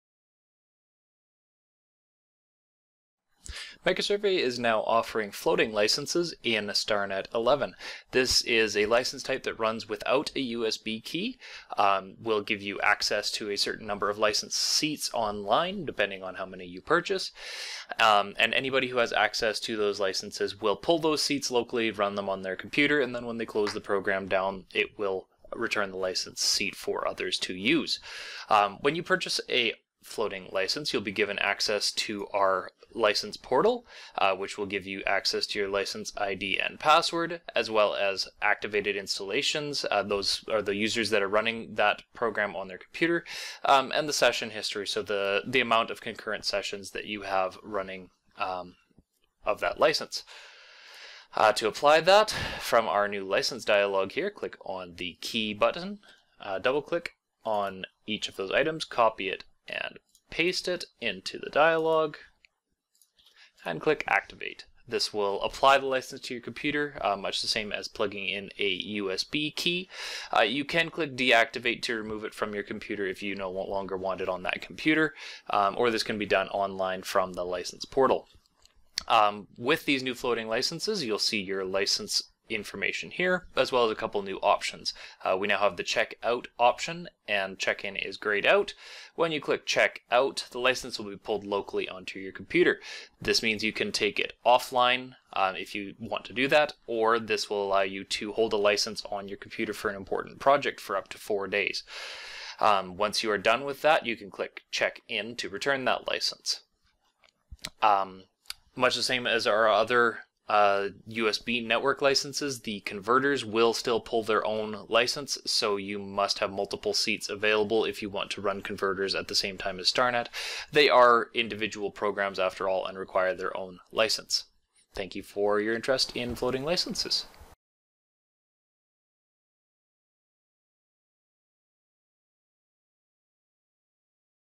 STAR*NET 11 Upgrade Tour - Floating Licenses
A new floating license type has been added to STAR*NET 11. This license type does not require a USB key, and is available while connected to the internet.